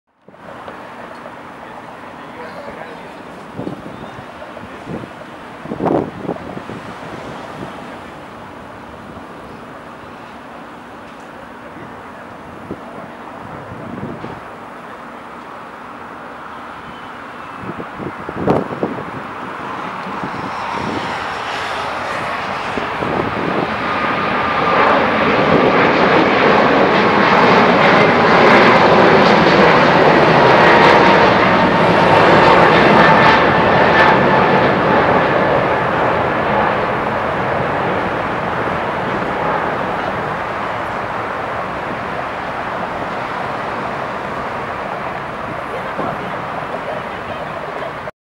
Transaero Boeing 777-200ER [EI-UNX] Departing LAX
Here is a Transaero Boeing 777-200ER departing LAX runway 25R.

Boeing 777-222ER

*Nikon D5200 + Nikkor 70-300mm VR*